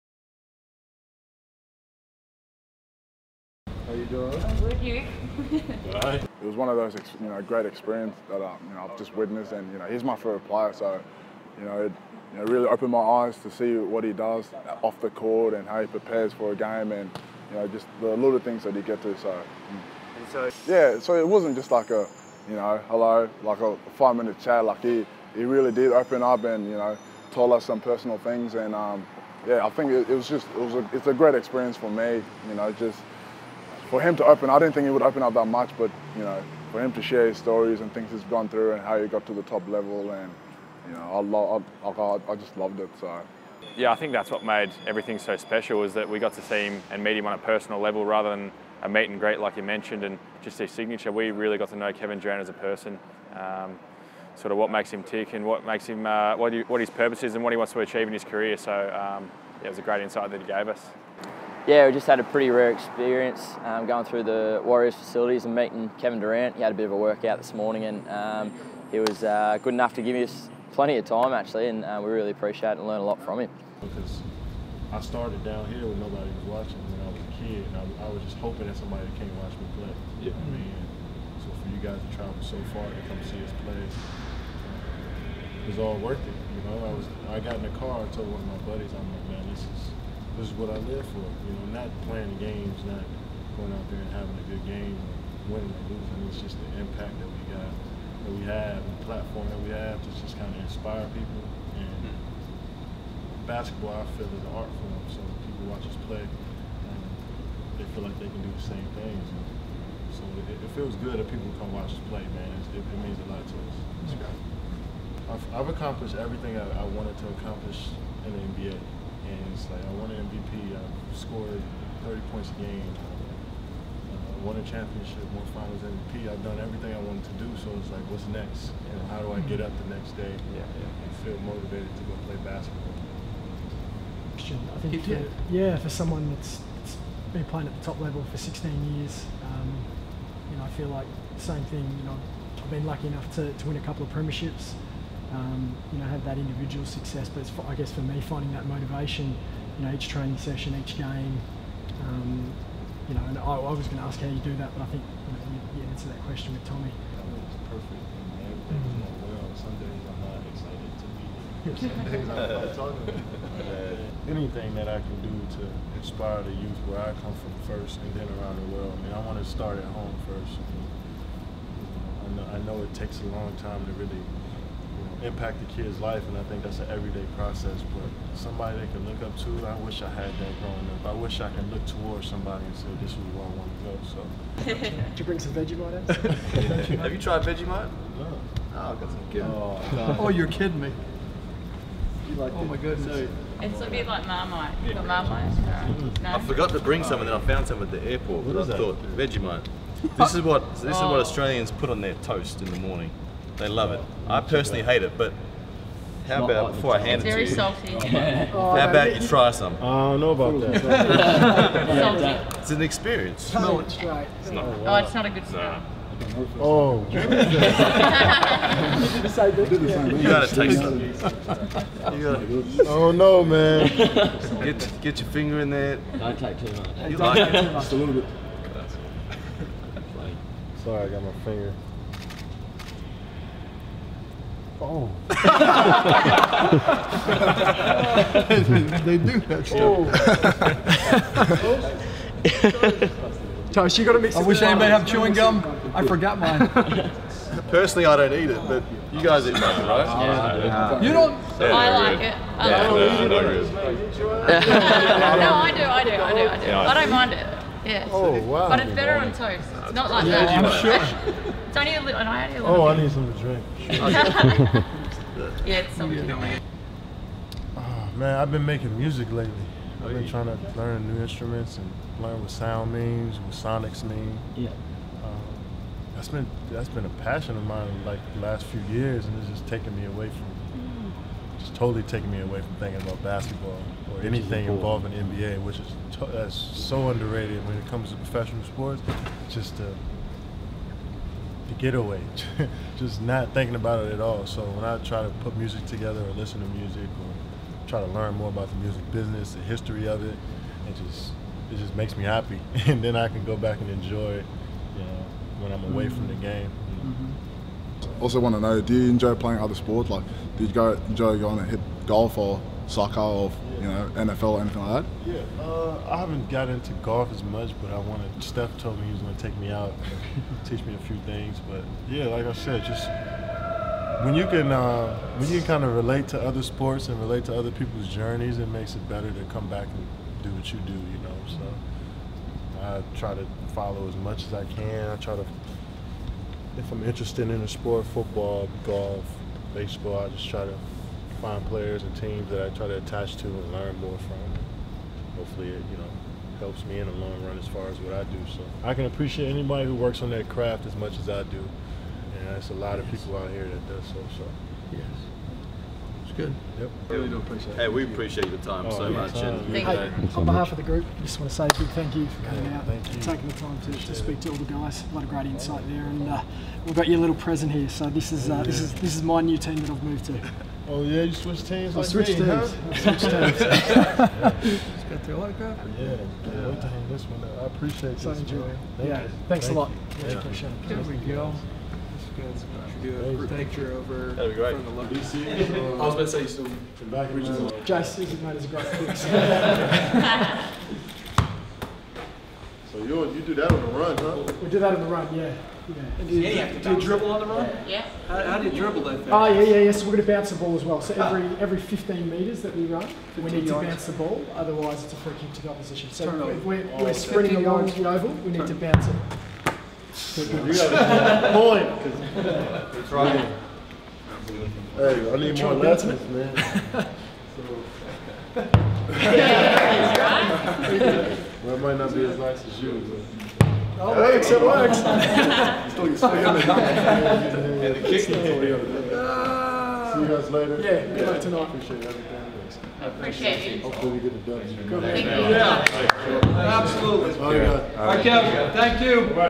'I Wish I Had That Growing Up' - Kevin Durant, presented by Hype Tours
A number of the AFL’s most high profile players, including Hawthorn’s Tom Mitchell and publisher Gary Ablett, had the opportunity to travel to the United States to witness and learn how the best of the best in world sport – prepare, train and lead their day-to-day lives.

NBA megastar Kevin Durant was among the best athletes in the world to feature in the Heart of Champions mini-documentary, presented by ExclusiveInsight.

Hosted by Hype Tours, the visit was designed to inspire, motivate and elevate the AFL travelling party.

*Courtesy credit to “NBA Entertainment” for providing the licensee the right to publish and broadcast footage captured for the program.